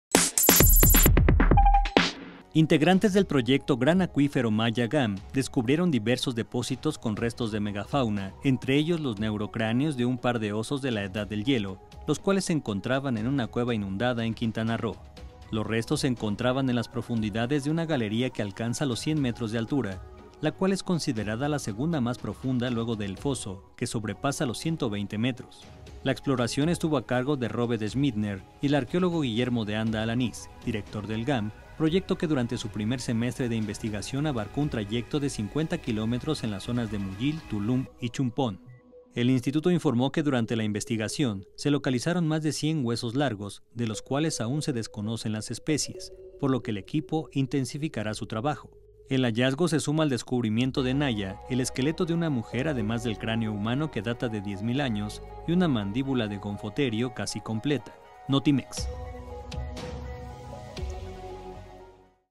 Proyecto acuífero descubre megafauna de la Edad de Hielo
Los restos se encontraban en las profundidades de una galería que alcanza los 100 metros de altura en Quintana Roo